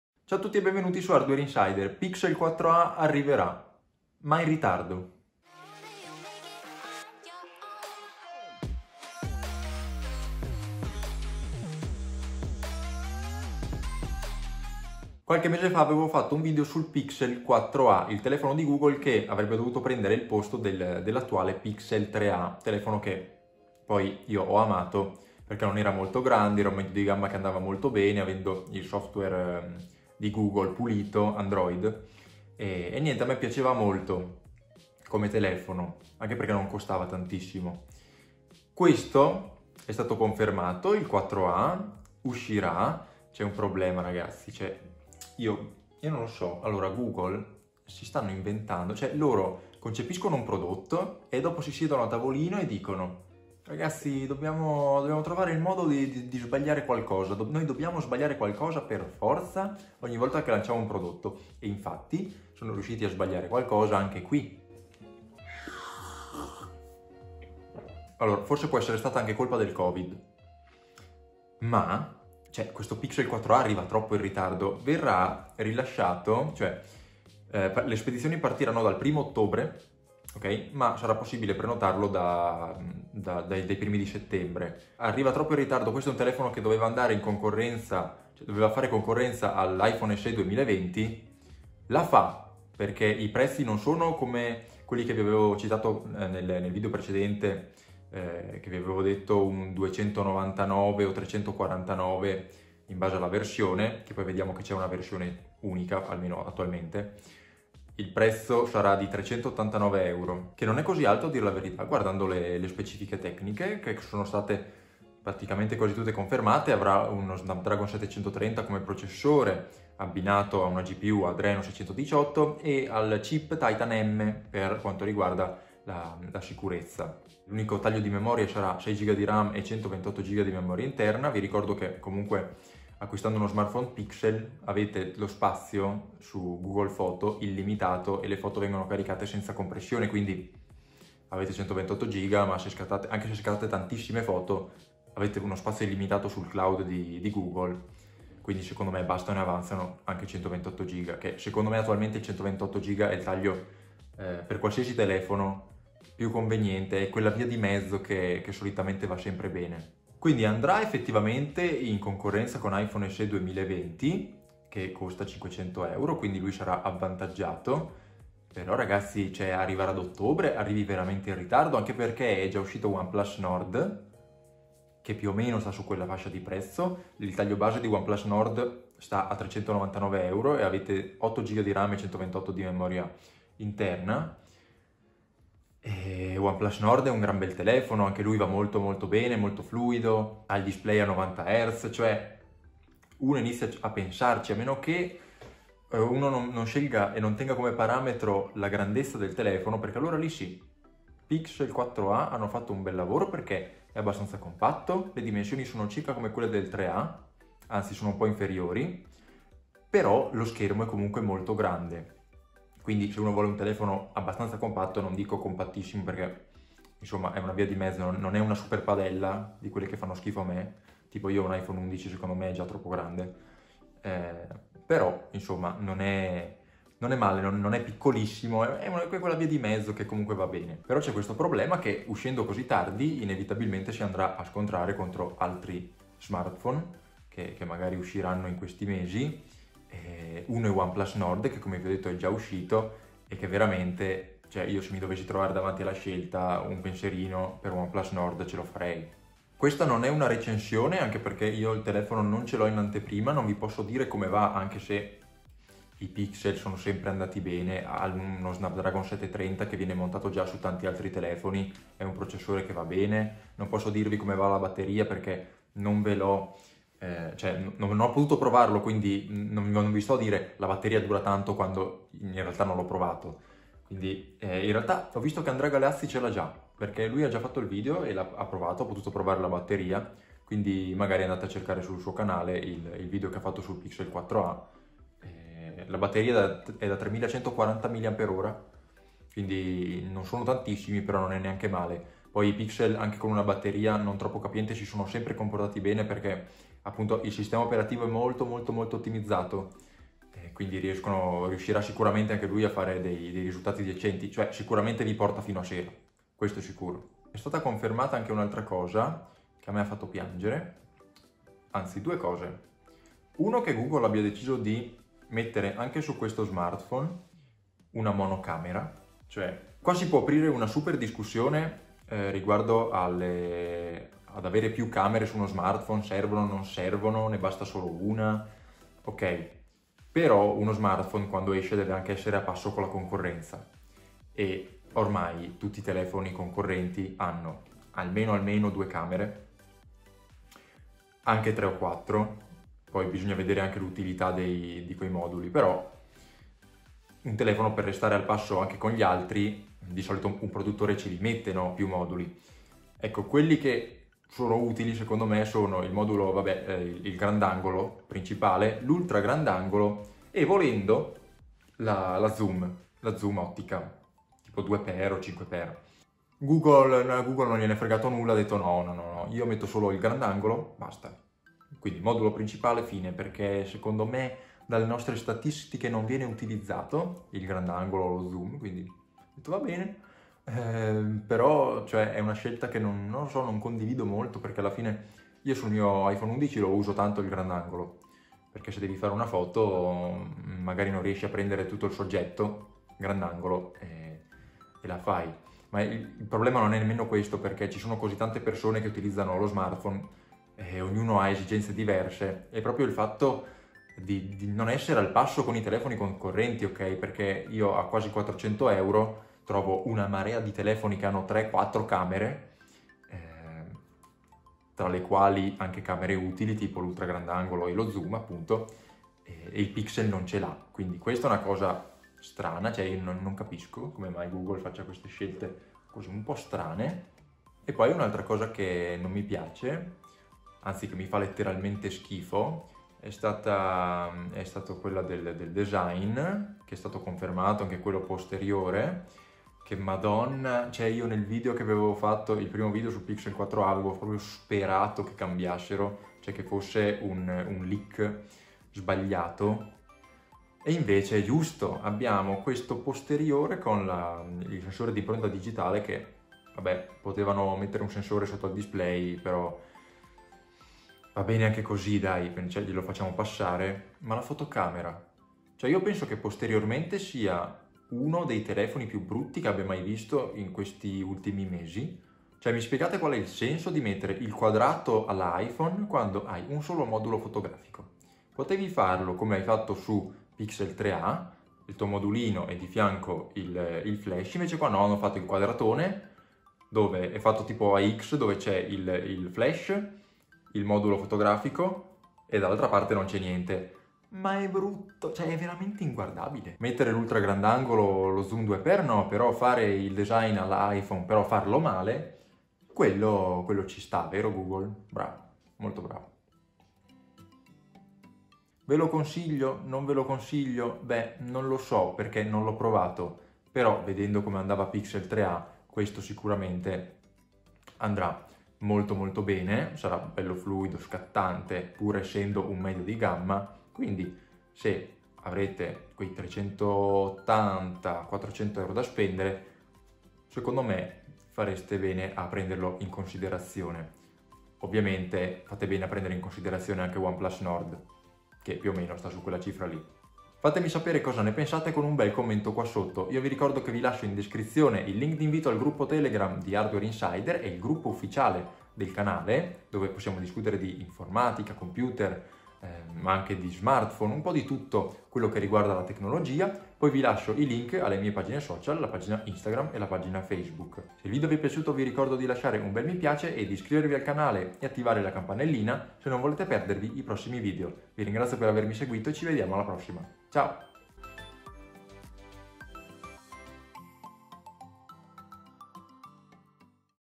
PIXEL 4A È UFFICIALE, MA ARRIVA IN RITARDO
Google mostra finalmente Pixel 4A, il successore dell'ottimo Pixel 3A dell'anno scorso.
C'è un solo problema: arriverà ad Ottobre, troppo in ritardo rispetto alla concorrenza.